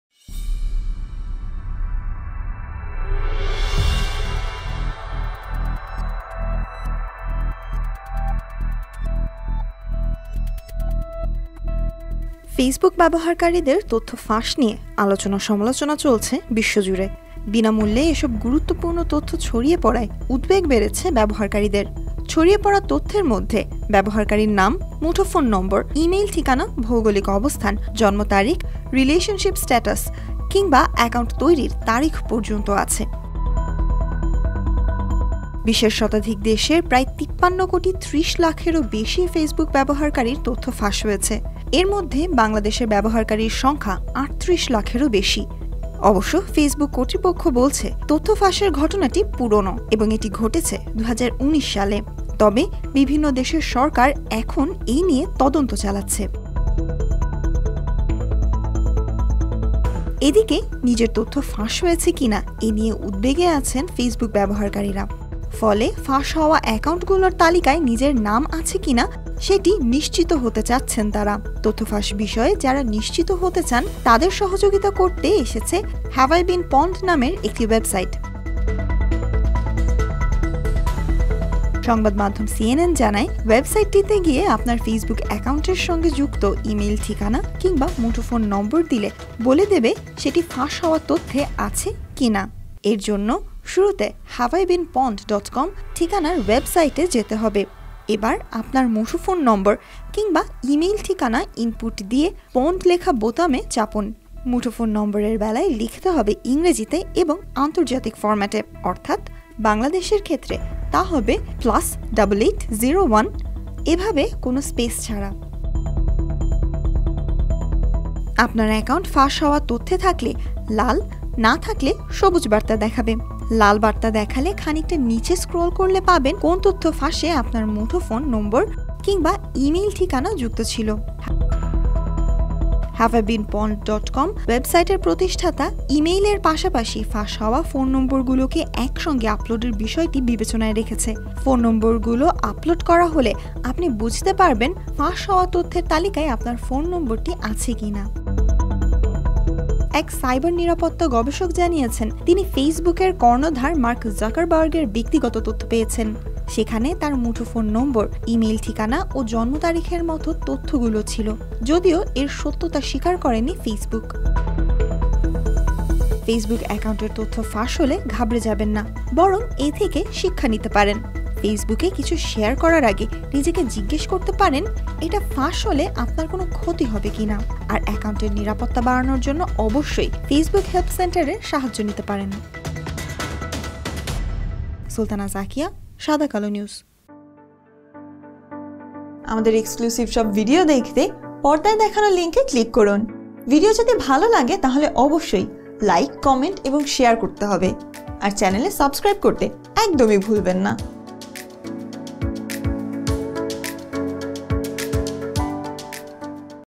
ফেসবুকে আপনার তথ্য ফাঁস হয়েছে কিনা জানবেন যেভাবে | সাদাকালো নিউজ | sadakalo news
ফেসবুক ব্যবহারকারীদের তথ্য ফাঁস নিয়ে আলোচনা চলছে বিশ্বজুড়ে। সর্বশেষ বিশ্বের অন্তত ১০০টি দেশের প্রায় ৫৩ কোটি ৩০ লাখেরও বেশি গ্রাহকের ফেসবুক তথ্য ফাঁস হয়েছে। এর মধ্যে বাংলাদেশের ব্যবহারকারীর সংখ্যা ৩৮ লাখেরও বেশি।

নিজের তথ্য ফাঁস হয়েছে কিনা এ নিয়ে উদ্বেগে আছেন ফেসবুক ব্যবহারকারীরা। ফলে ফাঁস হওয়া অ্যাকাউন্টগুলোর তালিকায় নাম আছে কিনা সেটি নিশ্চিত হতে চাচ্ছেন তারা।  তথ্য ফাঁস বিষয়ে যারা নিশ্চিত হতে চান তাদের সহযোগিতা করতে এগিয়ে এসেছে ‘হ্যাভআইবিনপনড’ নামের একটি ওয়েবসাইট।